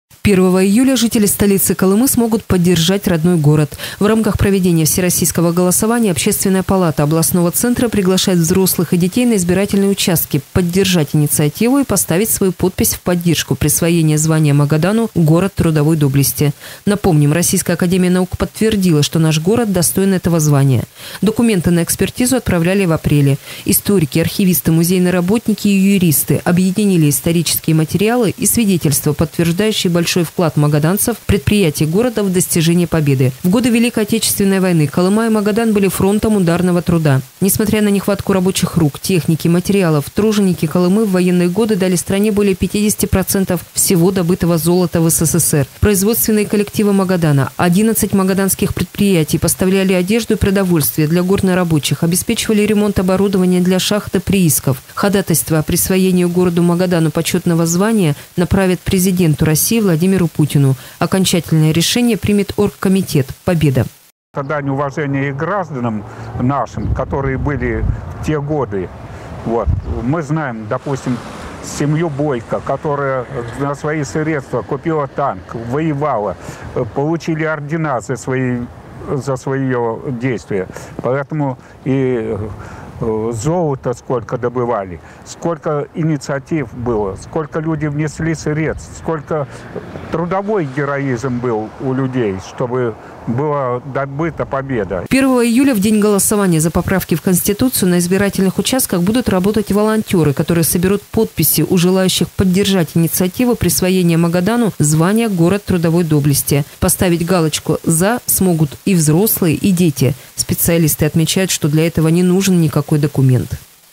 Поддержим любимый Магадан
Магадан 2020

1 июля жители столицы Колымы смогут поддержать родной город. В рамках проведения Всероссийского голосования, общественная палата областного центра приглашает взрослых и детей на избирательные участки поддержать инициативу и поставить свою подпись в поддержку присвоения звания Магадану «Город трудовой доблести». Напомним, Российская академия наук подтвердила, что наш город достоин этого звания. Документы на экспертизу отправляли в апреле. Историки, архивисты, музейные работники, юристы объединили исторические материалы, свидетельства, подтверждающие большой вклад магаданцев и предприятий города в достижение Победы. В годы Великой Отечественной войны Колыма и Магадан были фронтом ударного труда. Несмотря на нехватку рабочих рук, техники, материалов, труженики Колымы в военные годы дали стране 55,5% всего добытого золота в СССР. 11 магаданских предприятий – поставляли одежду и продовольствие для горнорабочих, обеспечивали ремонт оборудования для шахт и приисков. Ходатайство о присвоении городу Магадану почетного звания «Город трудовой доблести» направят Президенту РФ Владимиру Путину. Окончательное решение примет Российский оргкомитет «Победа». Валерий Булынин : «Это дань уважениям гражданам нашим тех годов. Мы знаем семью Бойко, которая за свои средства купила танк, воевала, получила ордена. Золота сколько добывали, сколько инициатив было, сколько внесли средств. Трудовой героизм был у людей, чтобы была добыта Победа».  1 июля, в День голосования за поправки в Конституцию, на избирательных участках будут работать волонтеры, которые соберут подписи у желающих поддержать инициативу присвоения Магадану звания «Город трудовой доблести». Поставить галочку «За» смогут и взрослые, и дети. Специалисты отмечают, что для этого не нужен никакой документ.